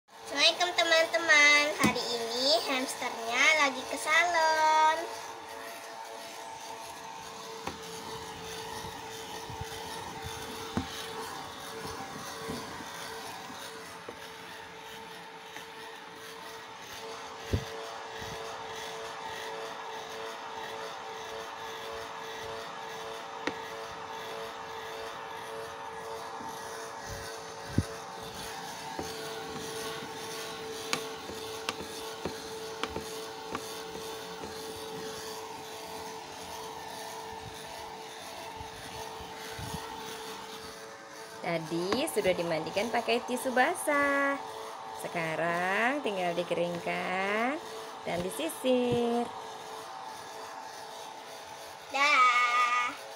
Anak hamster ke salon#shorts
Hamster lahirkan 12 anak. Sekarang anak2 ya sudah umur 6mgg. Karena kandang kecil jadinya pada bertumpuk dan kotor,  jadi harus rajin nyaman biar pd bersih...